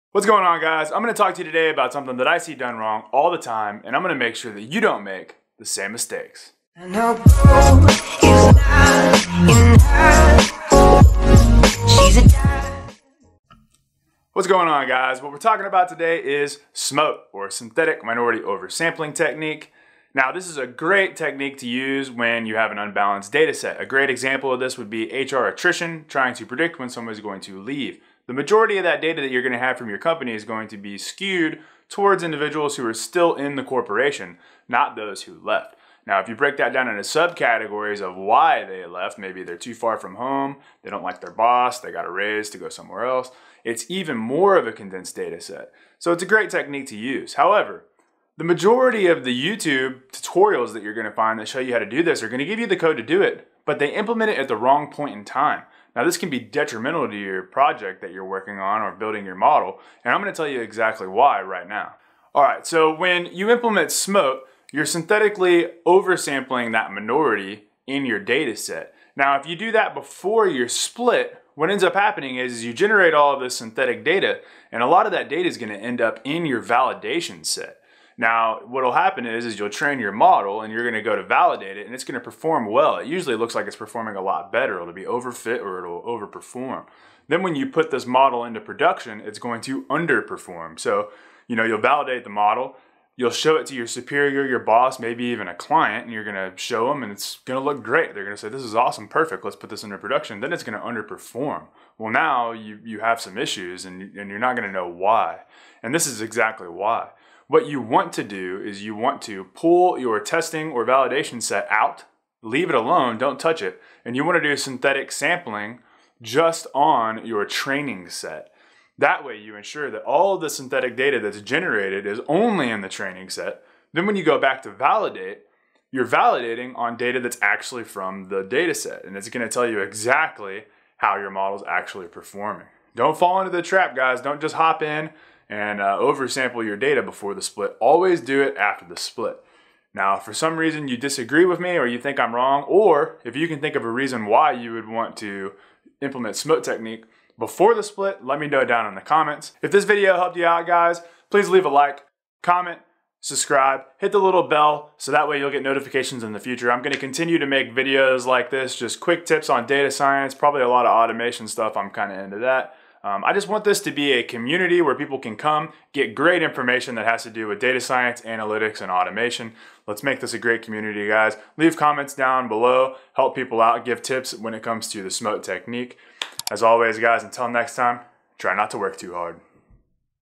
SMOTE - SYNTHETIC MINORITY OVERSAMPLING TECHNIQUE | OVERSAMPLING MACHINE LEARNING (TIPS & TRICKS)
SMOTE or Synthetic Minority Oversampling Technique can be an excellent machine learning method for dealing with unbalanced data sets. However, I see a common mistake made frequently when oversampling and with these machine learning tips and trick, you can avoid the headaches.